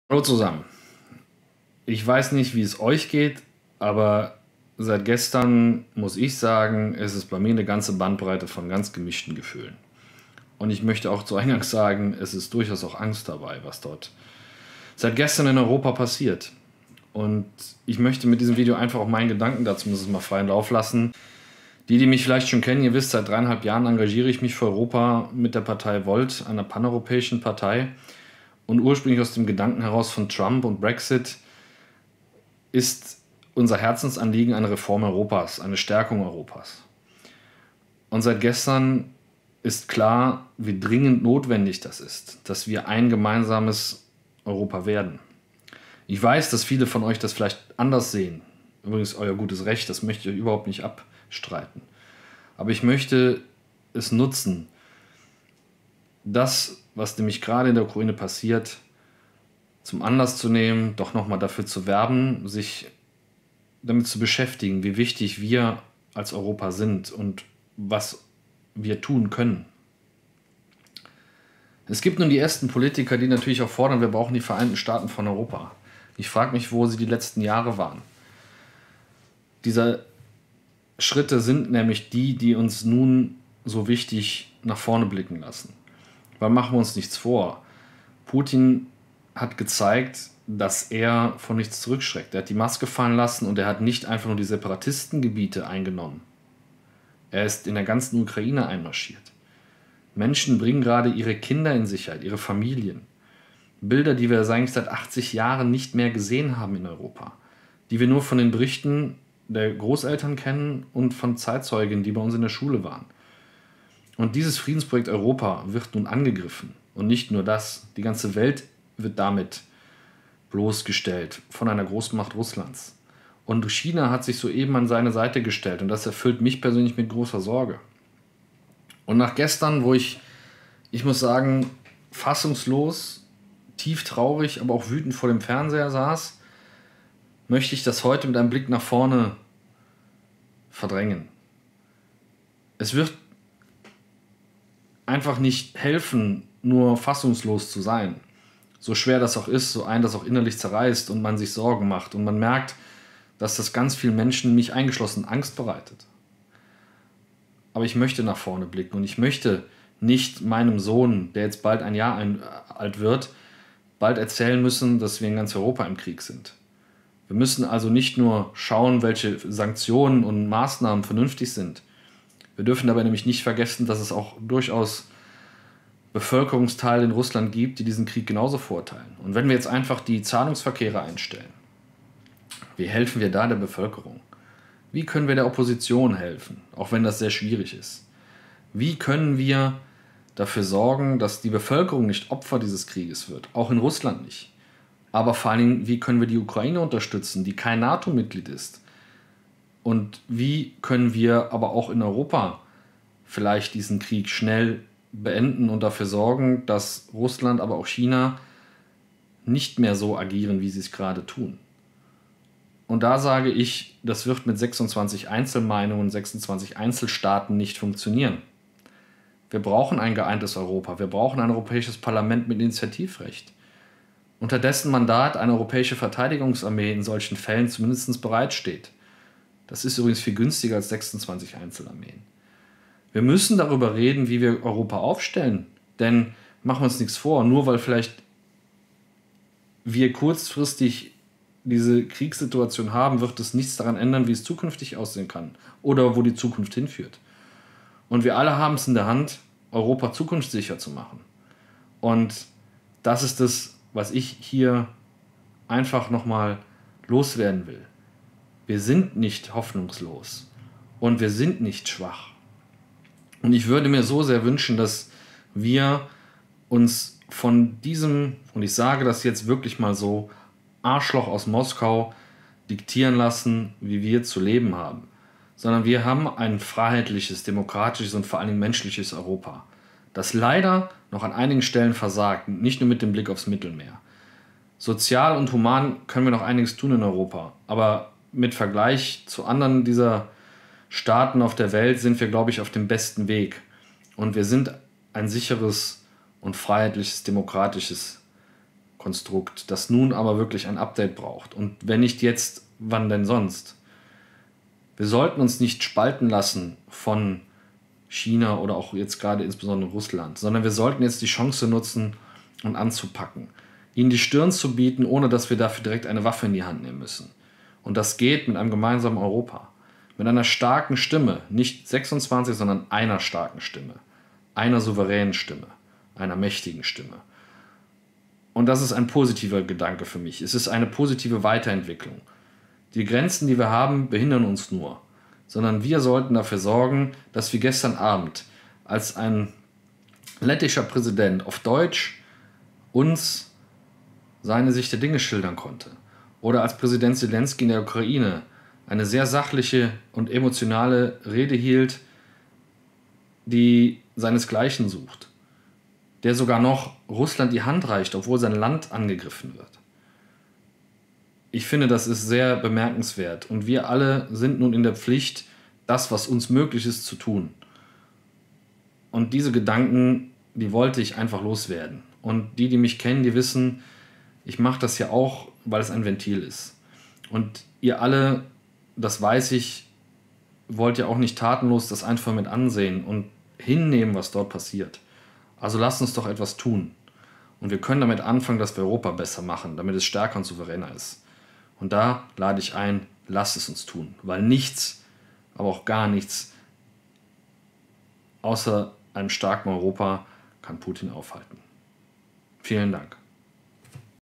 2022 Souveräne EU nötig
Europa muss Putin die Stirn zu bieten. Dazu muss die EU / Europäische Union souveräner und geschlossener werden. 26 einzelne Meinungen und Stimmen sind keine Option für die Zukunft.